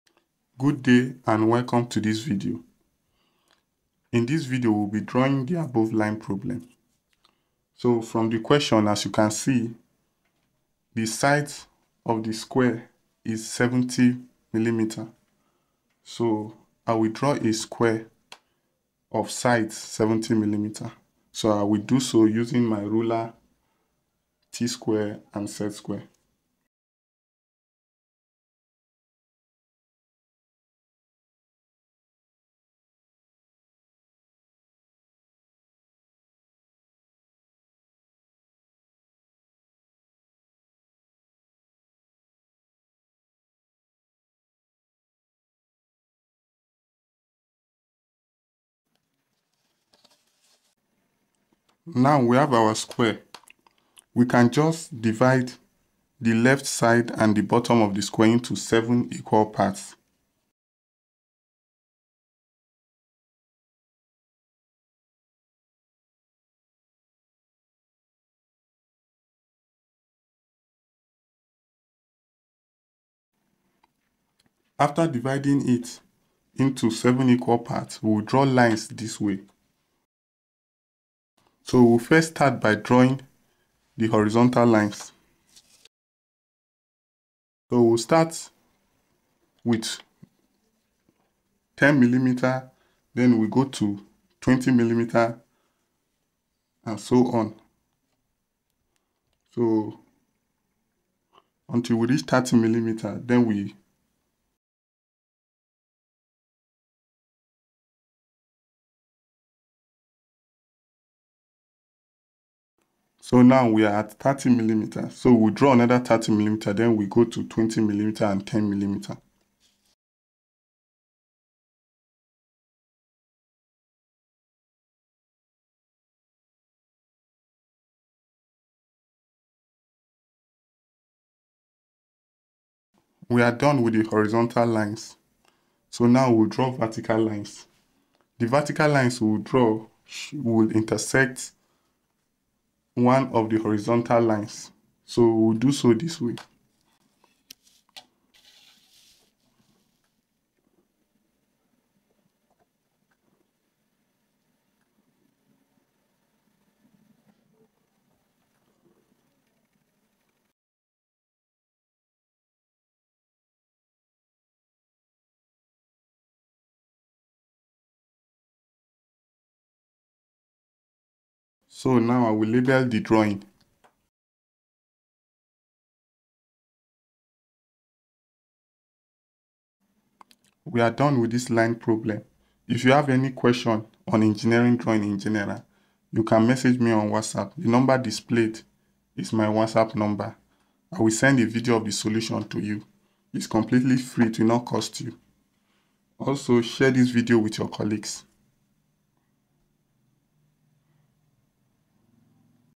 LINE PROBLEM 2 || HOW TO DRAW LINE PROBLEM 2
This video explain how to draw the above line problem which is line problem 2 from Engineering Drawing 1 by M. A Parker and F. Pickup.

lineproblem 2